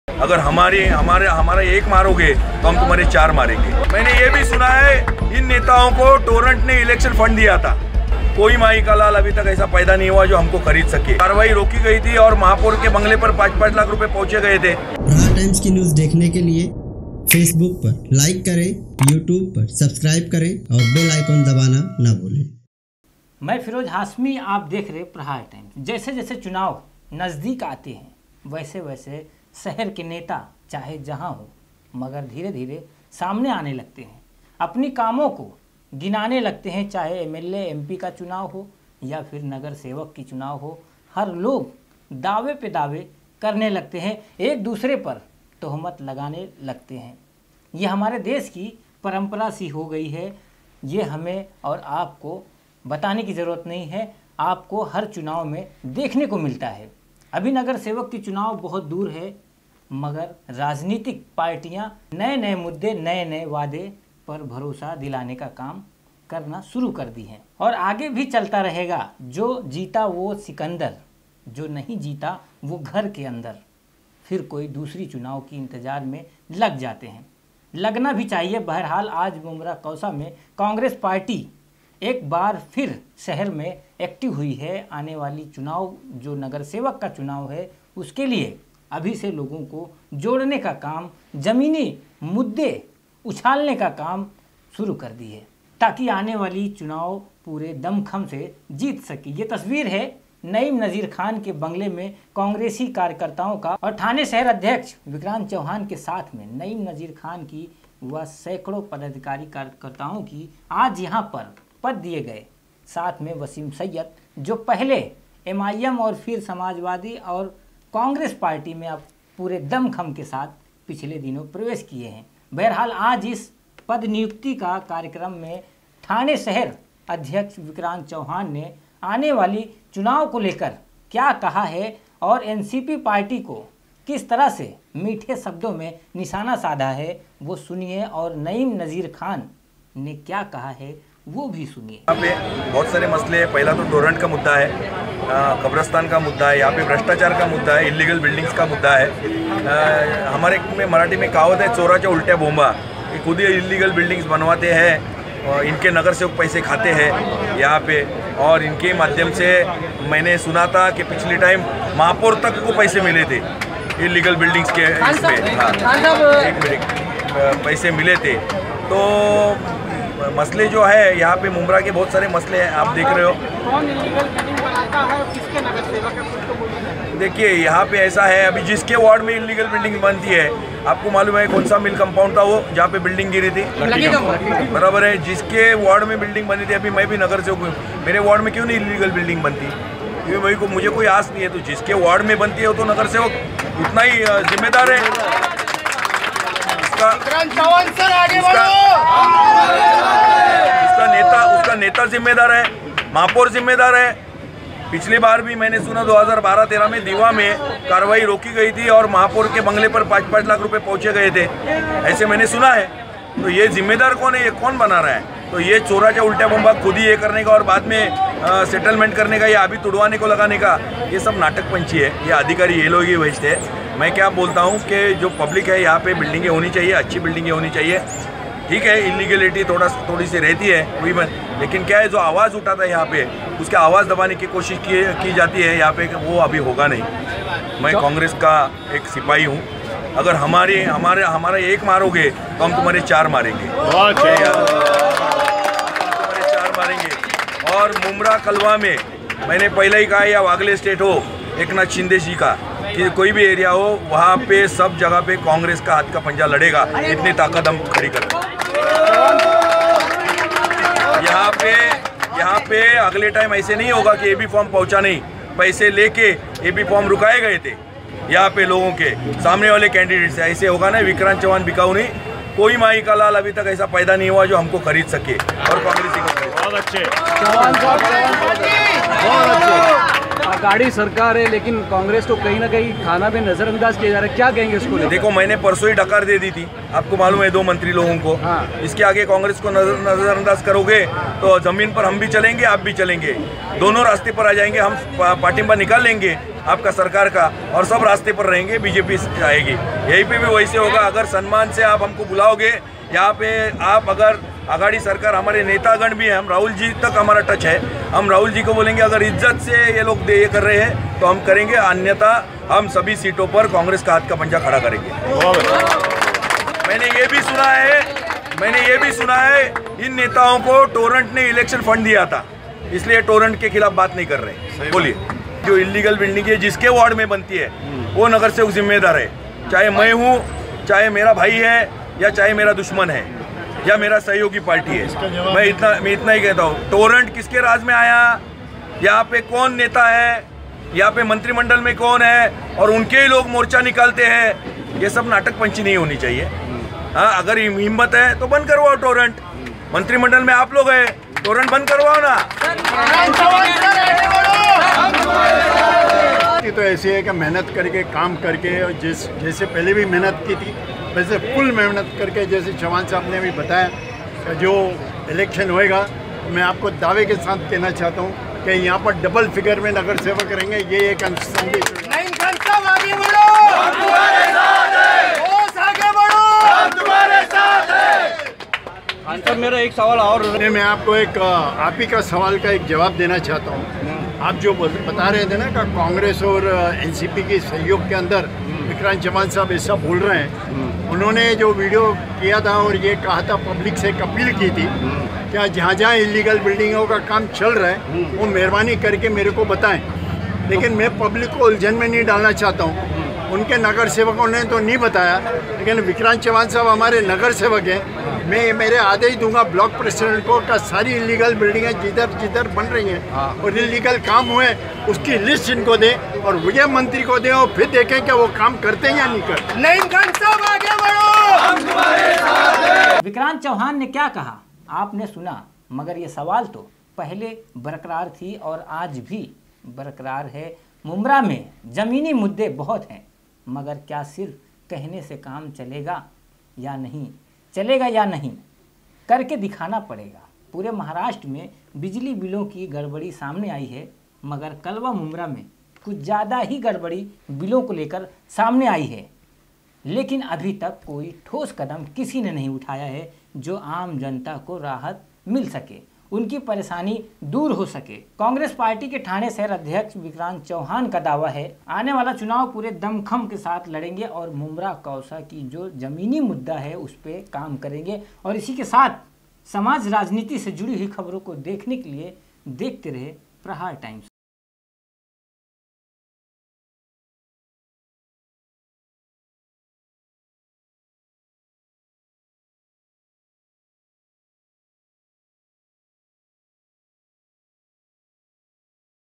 मुंब्रा में कांग्रेस पद नियुक्ति के दौरान विक्रांत चव्हाण विरोधियों को ललकारा | Prahar Times
मुंब्रा में कांग्रेस पद नियुक्ति के दौरान विक्रांत चव्हाण विरोधियों को ललकारा मुंब्रा की समस्याओं पर विक्रांत चव्हाण ने जमकर बोला और आने वाली नगरसेवक के चुनाव में पूरी तरह सीटों पर कांग्रेस लड़ेगी  ||